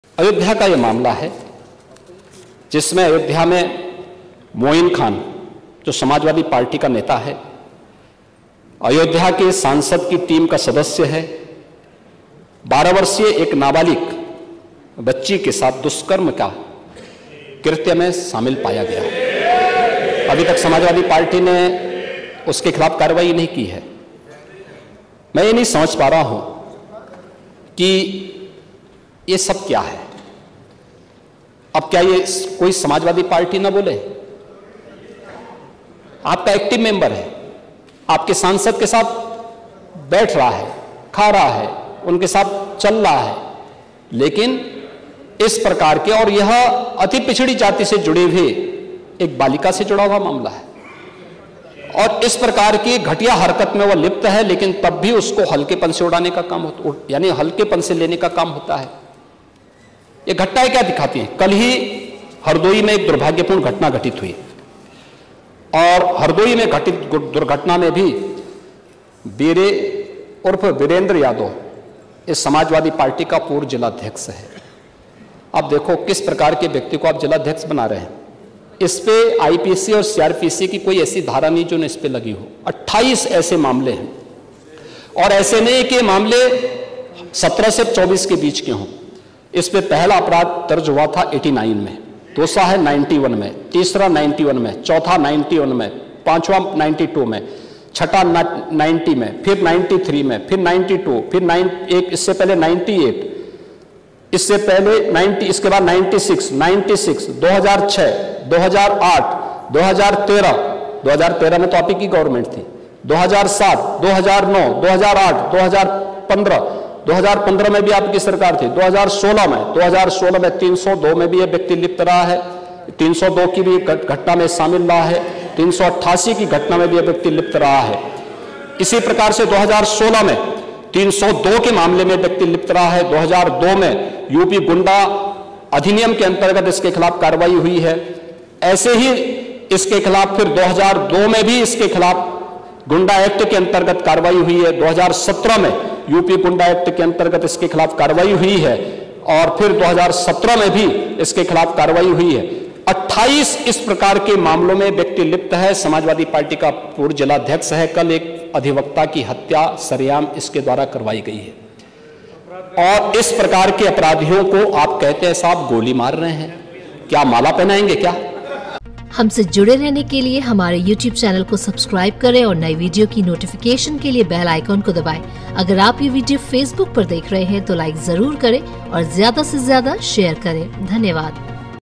‘इन अपराधियों को गोली ना मारे, तो क्या करें..’,अयोध्या में नाबालिग के साथ हुए दुष्कर्म पर भड़के Yogi
अयोध्या में नाबालिग के साथ हुए दुष्कर्म पर बोले CM योगी। घटना का जो अपराधी है, वो अयोध्या के सांसद के साथ रहता है। इन अपराधियों को गोली ना मारे, तो क्या माला पहनाएं। समाजवादी पार्टी ने अभी तक अपराधी के खिलाफ कार्रवाई नहीं की है।